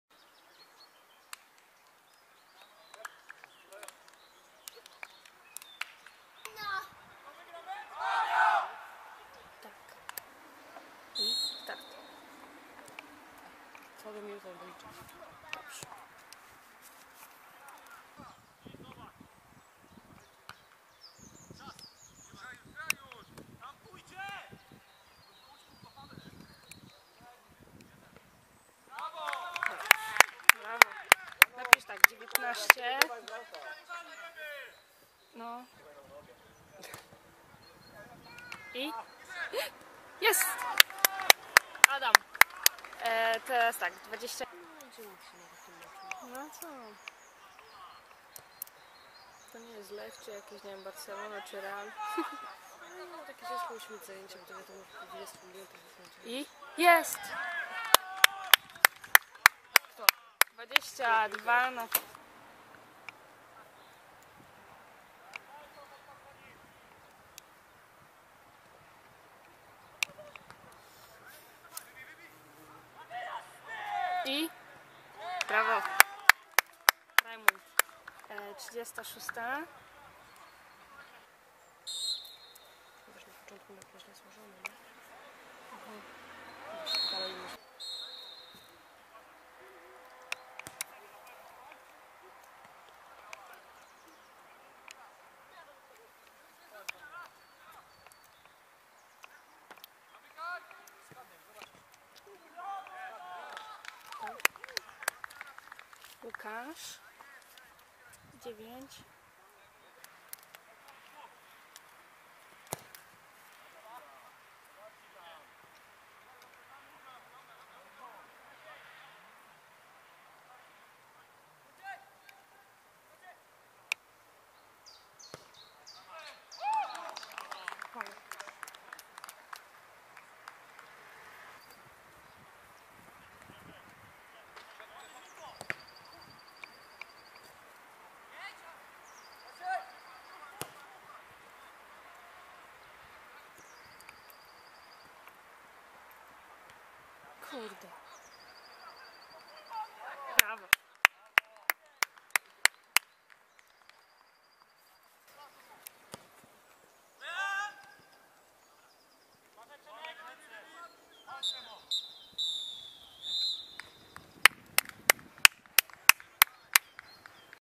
Avia II Kamionki 7-0 Okoń Sapowice - Bramki 26.04.15
15 Kolejka B-Klasy sezonu 2014/15
Avia II Kamionki 7-0 Okoń Sapowice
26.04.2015
Skład Avii:
Diego - J. Kwiatosz, P. Bocianowski, M. Lulka -, Ł. Kalwasiński, Ł. Kałużny, M. Matuszalski, R. Sobkowiak (67’ A. Jędrzejczak), A. Wiśniewski (61’ A. Seńczuk)  - A. Łapajczyk (46’ M. Libera),  M.Malicki (67’ D. Powierża)
Bramki:
1-0 M. Matuszalski 16’ (asysta: M. Malicki)
2-0 A. Wiśniewski 18’
3-0 A. Wiśniewski 19’ (asysta: P. Bocianowski)
4-0 R. Sobkowiak 33’ (asysta: M. Malicki)
5-0 A. Wiśniewski 55’ (asysta: Ł. Kalwasiński)
6-0 M. Matuszalski 80’ (asysta: M. Libera)
7-0 M. Matuszalski 87’